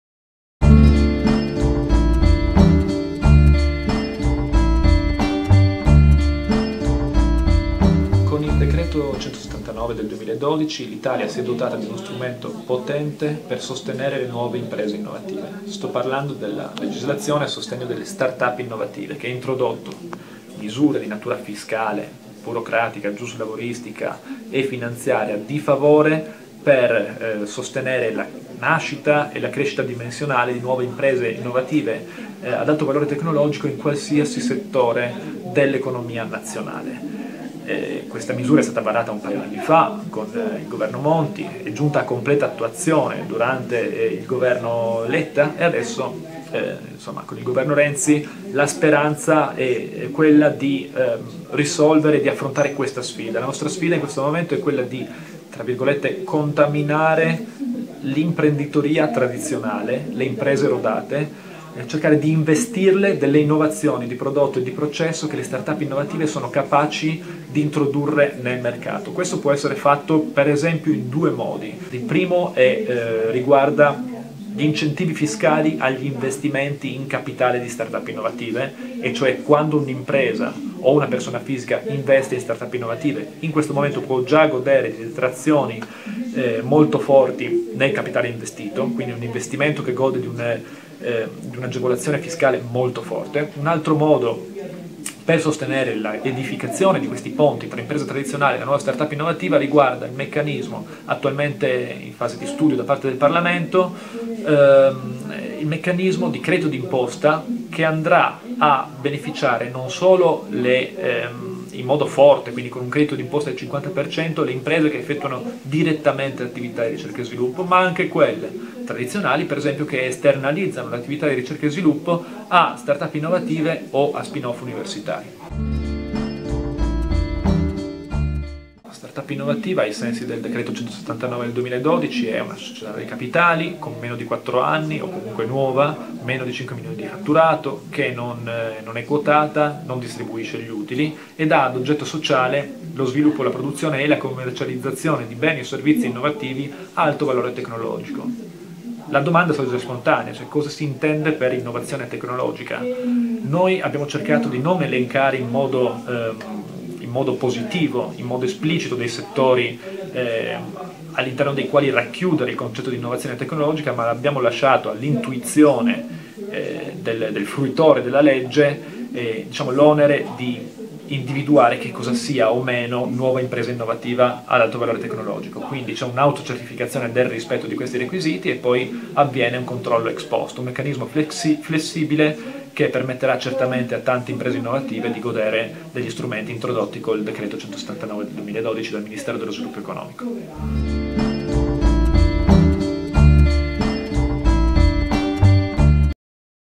FFF - Mattia Corbetta - Imprenderò 4.0
.@friuliforum In occasione di Imprenderò 4.0, a cura di Consorzio Friuli Formazione, Mattia Corbetta membro della Segreteria tecnica del Ministro per lo Sviluppo economico illustra le normative di legge per il sostegno alle imprese innovative, start-up.